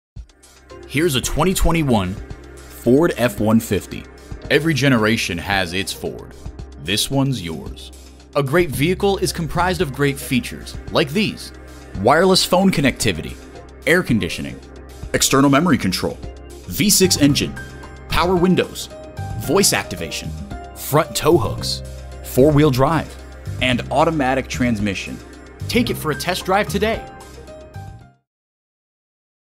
Used 2021 Ford F-150 Wausau, WI
Year: 2021
Make: Ford
Model: F-150
Trim: XL
Engine: 2.7L V6 EcoBoost
Transmission: 10-Speed Automatic
Color: Gray
Interior: Black
Mileage: 16761
Stock #: 1713P
VIN: 1FTEW1EP6MFA95554

Address:
2727 North 20th Ave